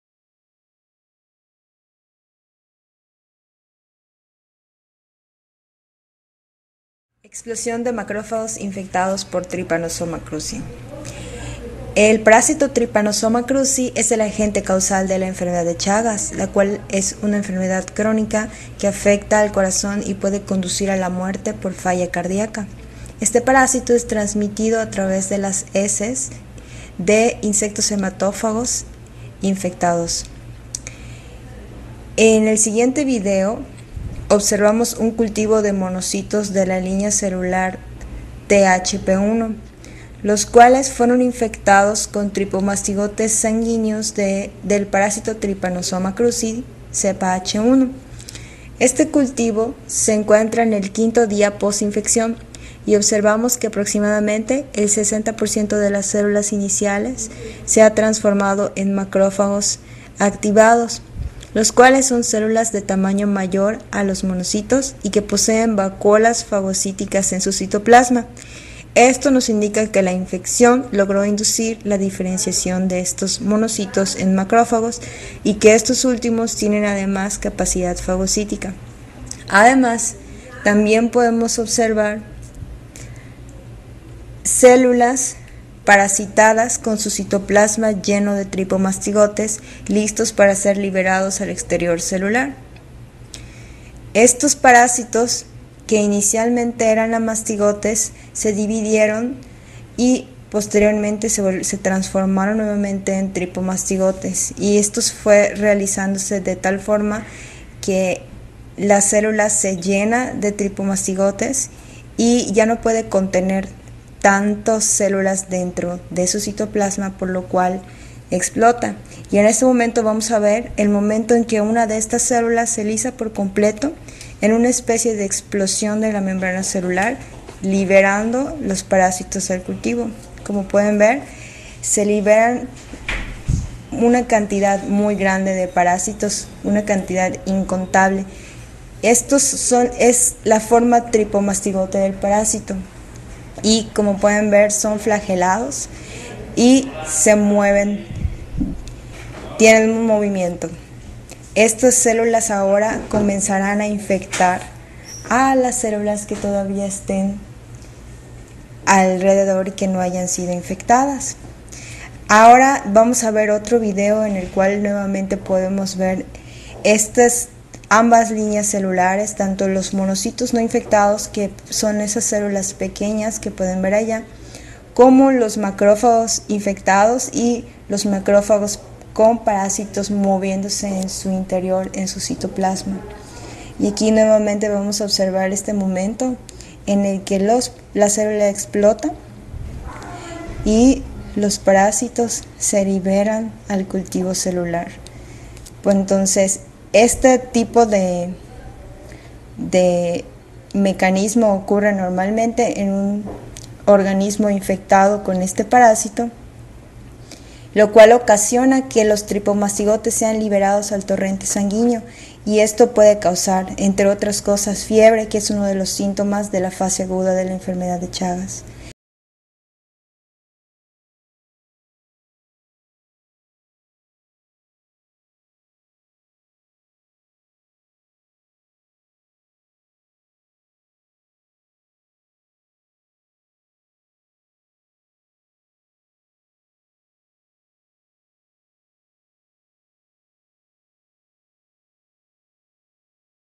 Película macrófagos
Este video narra el proceso de cultivo de células mononucleares, que al diferenciarse a macrófagos  e infectarse con Trypanosoma cruzi, se saturan de estos y provocan la lisis, lo que lamamos en este video "explosión de macrófagos infectados".

Para mayor información comunicarse al laboratorio de parasitología del CIR Biomedicas de la UADY.